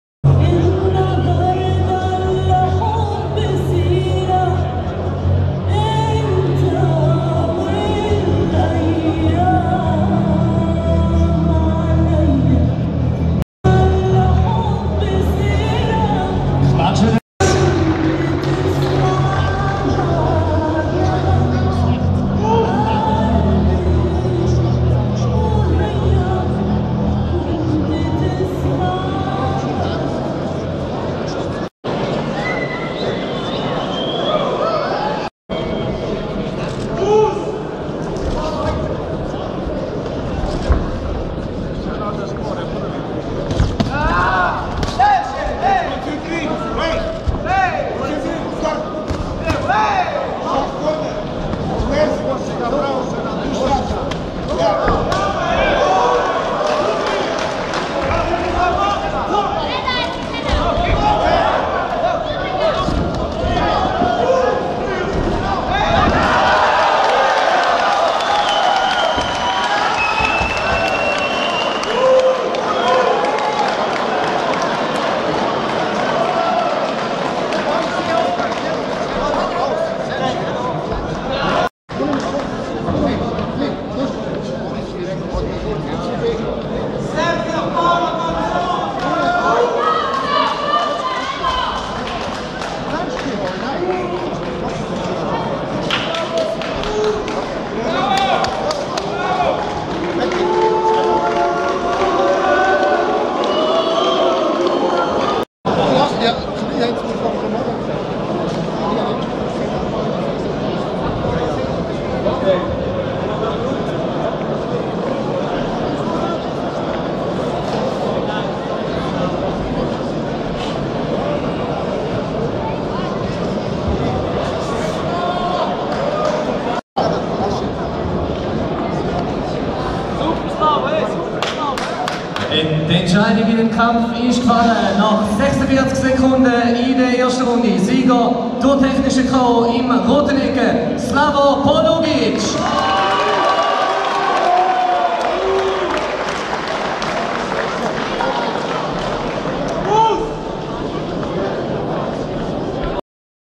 Slavo "The Iron Nee" Polugic VS Senad Hadzic / Round 1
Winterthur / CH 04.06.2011
W.F.C. Prestige Fight +96 Kg